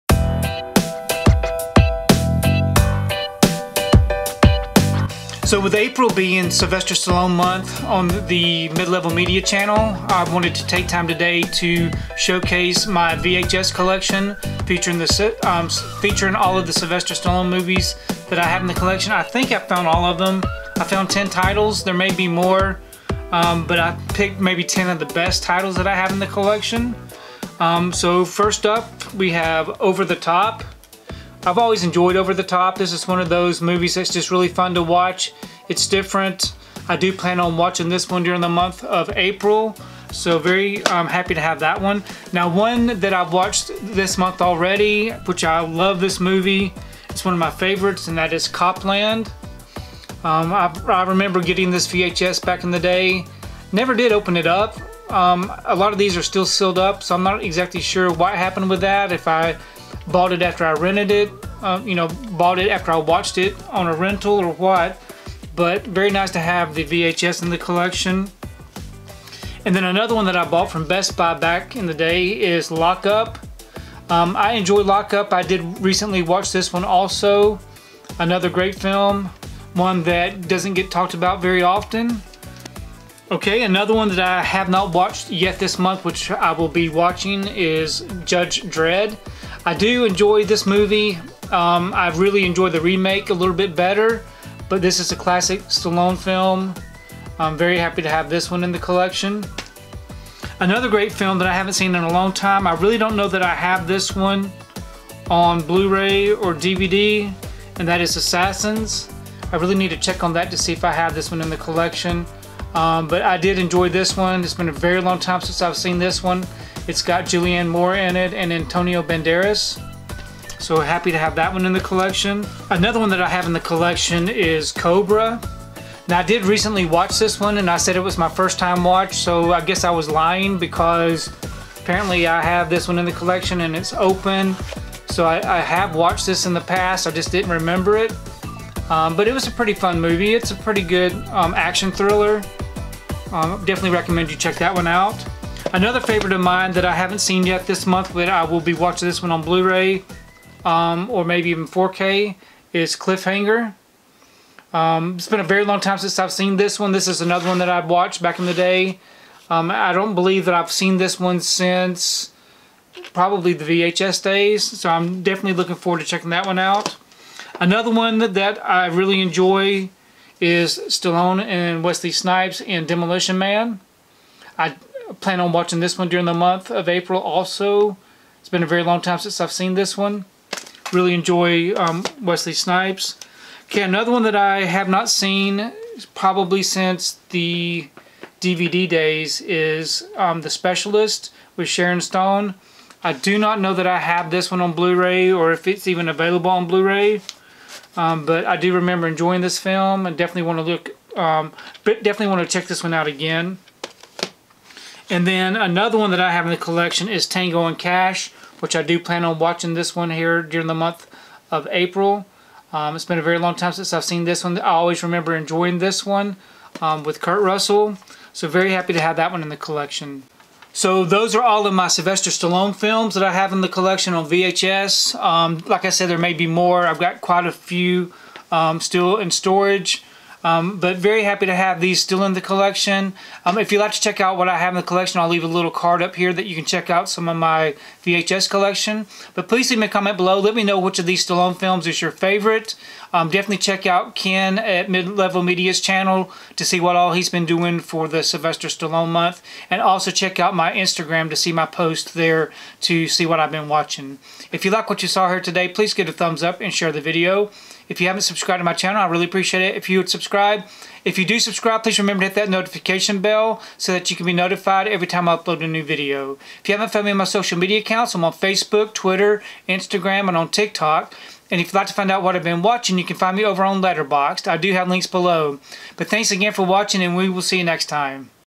My Top-10 Sylvester Stallone Movies on VHS
Check out these great Sylvester Stallone films that I have in my VHS Collection!